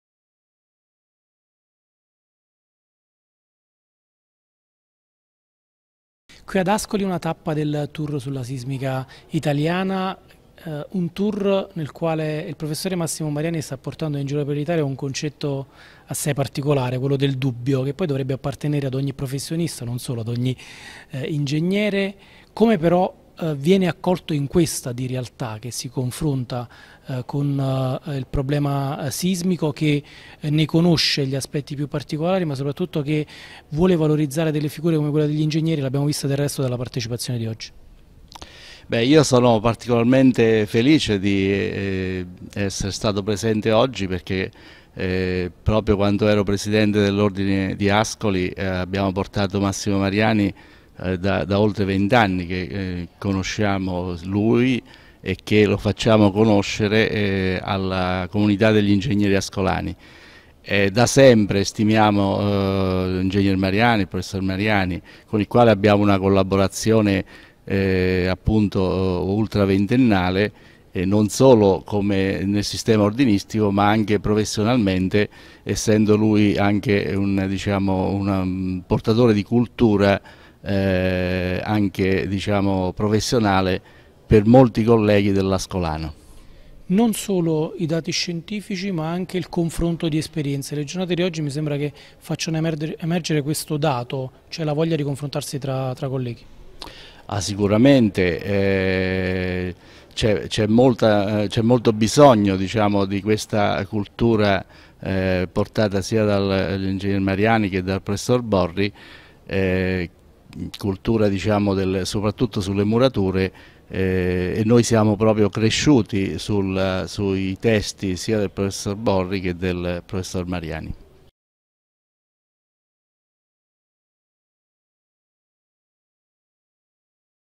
Occorre cultura tecnica per poter affrontare il problema del miglioramento sismico
Intervista dell'Ing. Pasquale Ubaldi, già Presidente dell'Ordine negli anni scorsi, durante la tappa di Ascoli del "La progettazione nasce dalla conoscenza del danno del sisma - Un viaggio nei luoghi dei più grandi terremoti italiani"